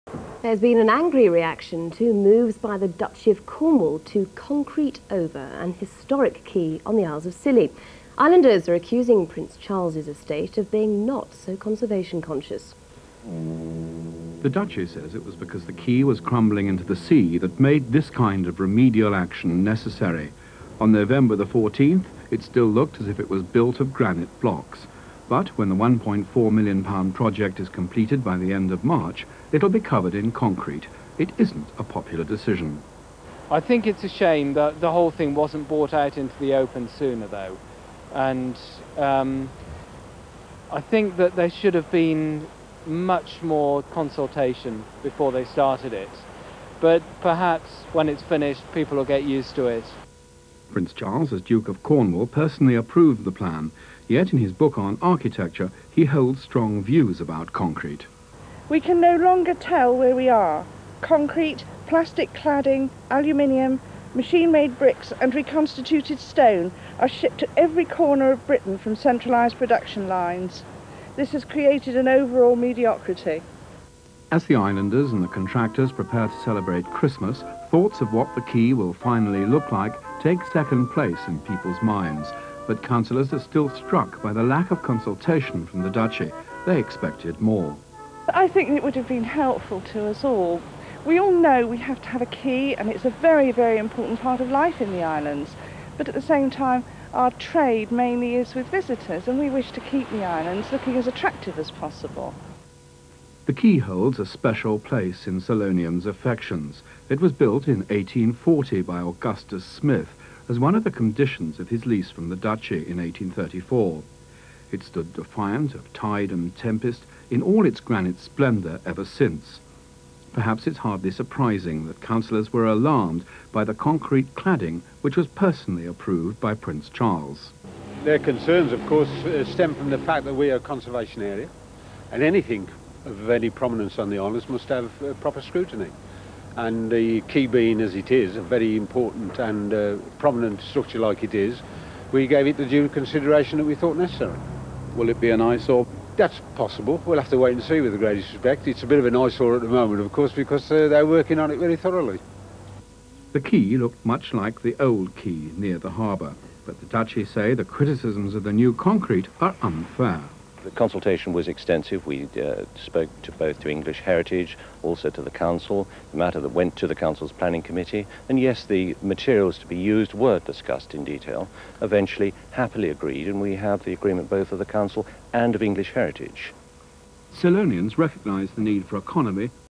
1994, News Report, Quay Upgrades to St Mary's.
Filmed on Scilly and Aired on ITV in 1994 here is a short news report on the Quay Upgrades.

0:35 - Interview with Keith Buchanan
1:14 - Music by Wayside Music
1:30 - Interview with Sue Pritchard
2:09 - Interview with Pat Greenlaw

Scilly lovers, everywhere, welcome.